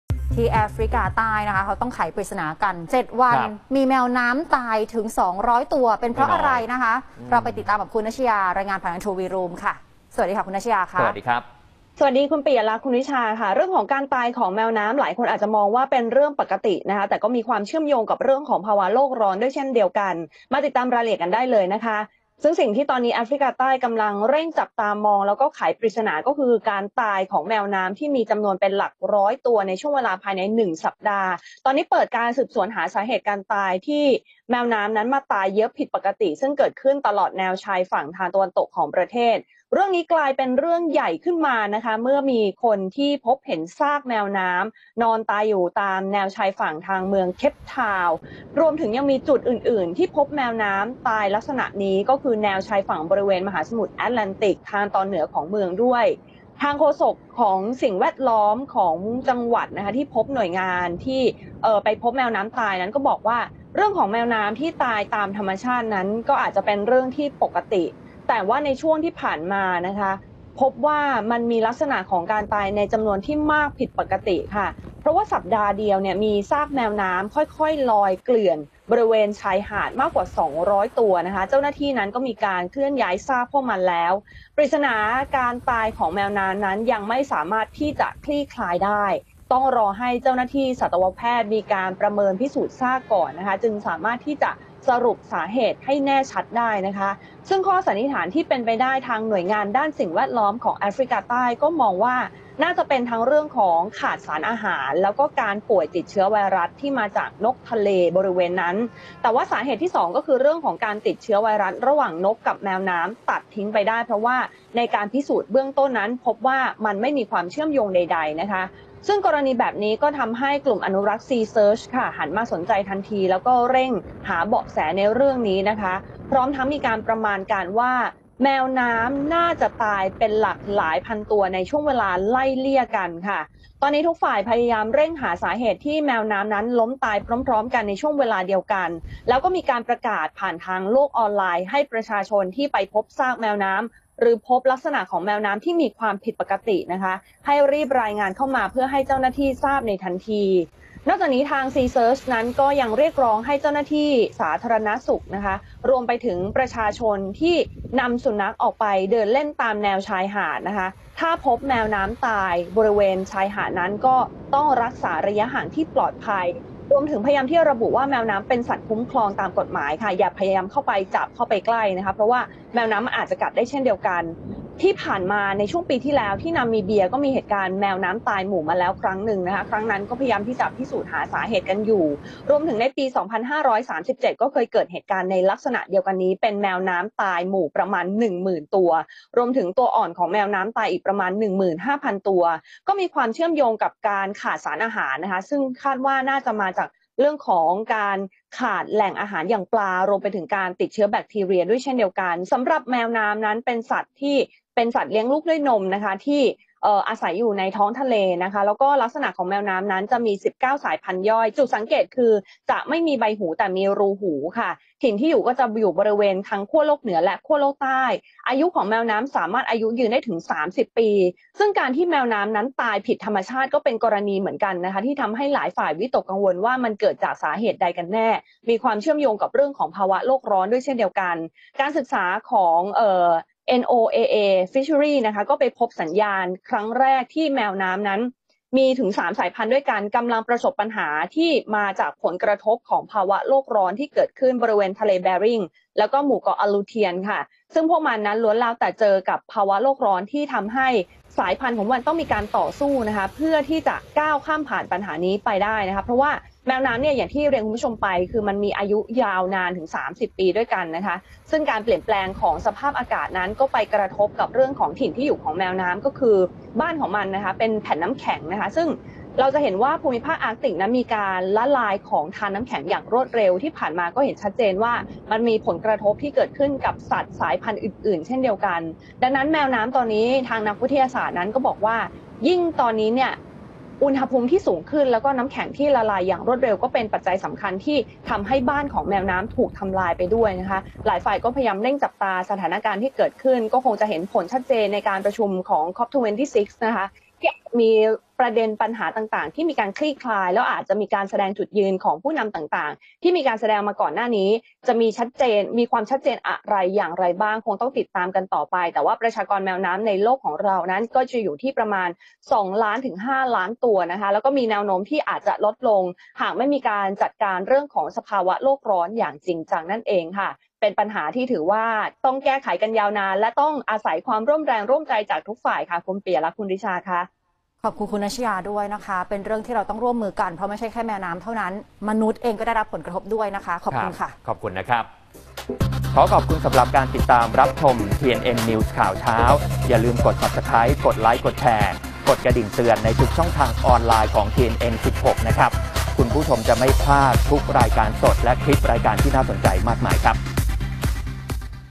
VROOM : ไขปริศนา 7 วัน !!! แมวน้ำ 200ตัว เกยตื้นตาย l TNN News ข่าวเช้า l 10-11-2021
แมวน้ำแถบแอฟริกาใต้ ทยอยตายริมชายหาดมากกว่า 200 ตัว ทำให้รัฐบาลแอฟริกาใต้เร่งสอบสวนหาสาเหตุการตายของแมวน้ำที่เยอะผิดปกติ ตลอดแนวชายฝั่งทางตะวันตกของประเทศ คาดว่ามาจากการขาดสารอาหาร และการติดเชื้อไวรัส แต่ปริศนาการตายของแมวน้ำยังไม่คลี่คลาย เพราะต้องรอให้สัตวแพทย์ทำการตรวจพิสูจน์ให้แน่ชัด ก่อนสรุปสาเหตุ